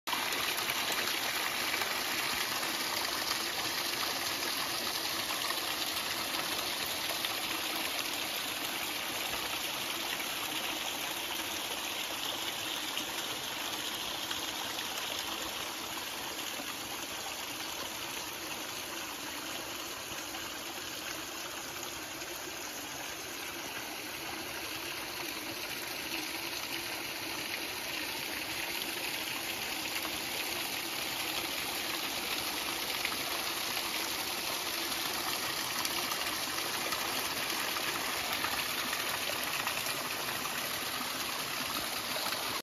The Mossy Spot. Northern Nova Scotia
Out on a little solo drift scouting for a place to start a bushcraft camp. Came upon a nice little Fairy hole.